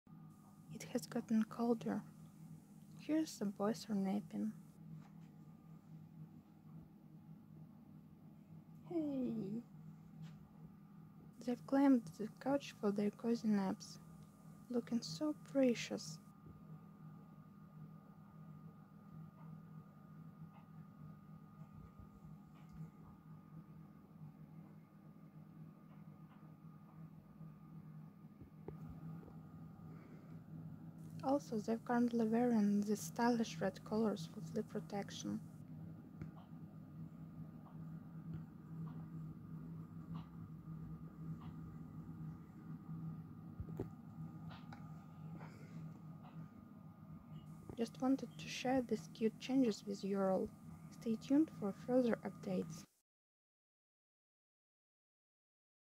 Cozy Cat Cuddles: Heartwarming Update
Hello everyone! It's has gotten colder so our boys now cuddling wile sleeping. That is so so adorable you need to see it!